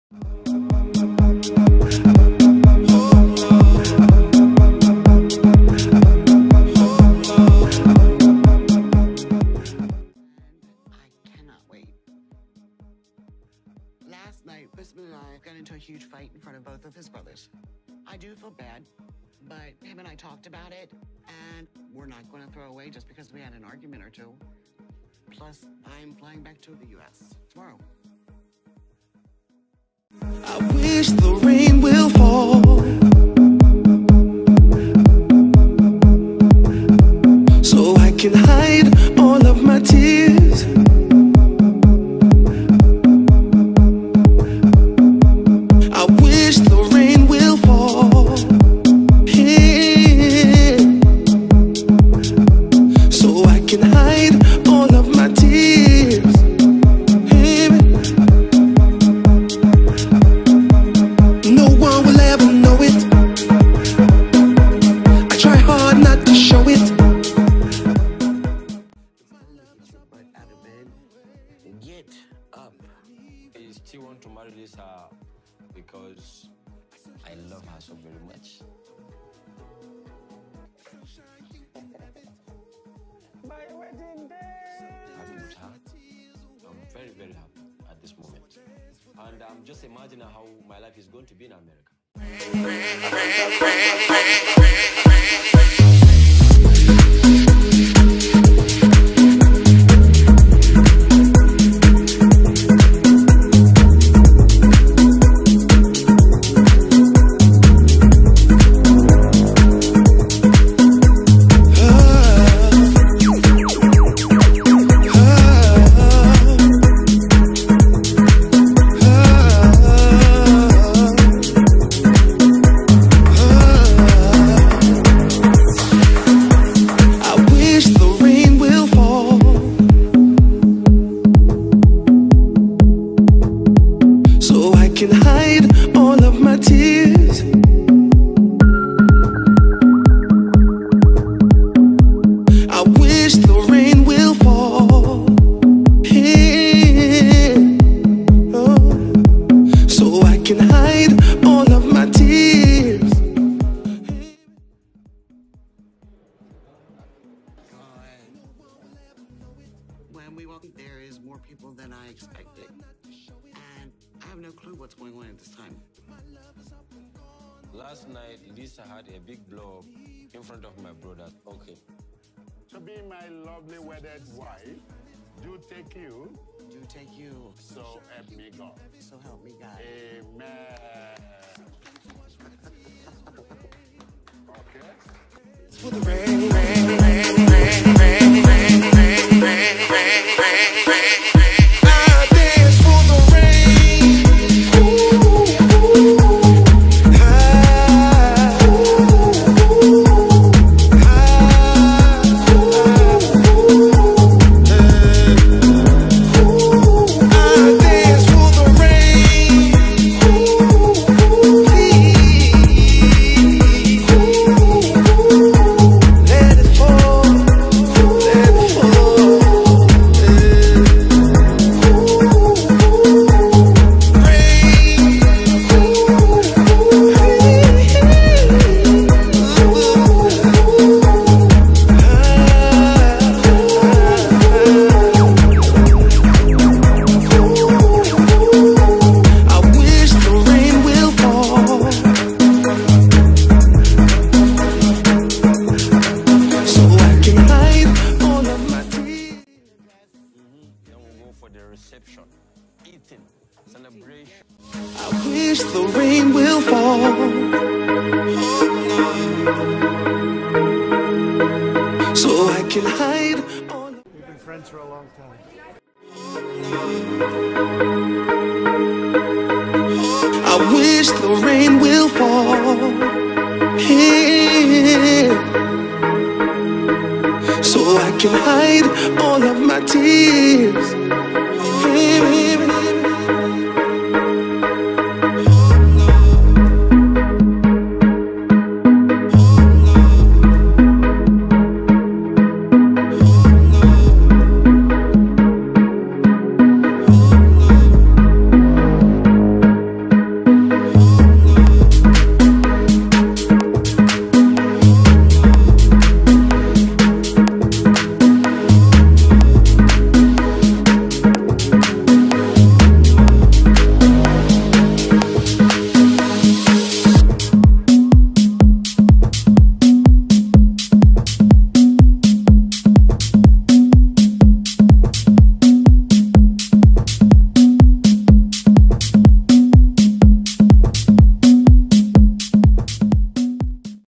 The Perfect Couple and The Wedding of The Century 😍 4K
Donald Trump (aka Lisa) marrying her long time friend Kanye West (aka Usman), the wedding of the century.
If you can't beat them, then at least laugh at them! 🤣
Please vote this come November! 👍🙂
deleteuber and @Keeptaxisalive

keep taxis alive
nigerian dance
african wedding dance
african funny dance